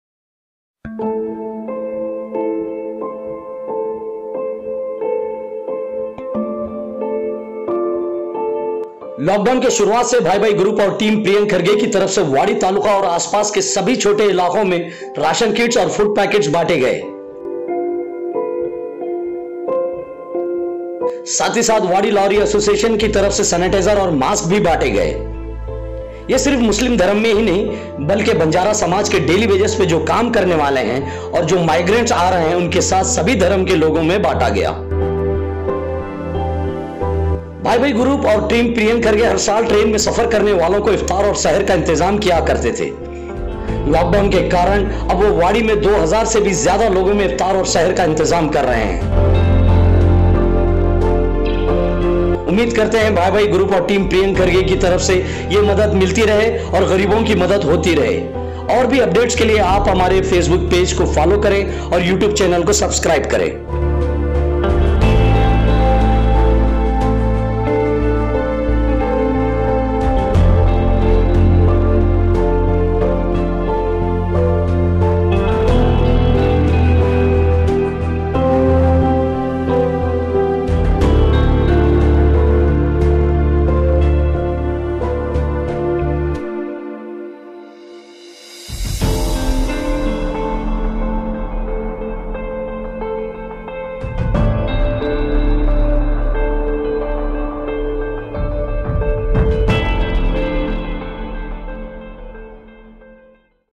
Bhai Bhai Group And Team Priyank Kharge Wadi
Gulbarga:- Bhai Bhai Group And Team Priyank Kharge Distributed Ration Kits,Food Packets Across Wadi And Also Lorry Association Distributed Sanitizer And Mask,Daily Arrangment Seher & Iftar For Fasting Purpose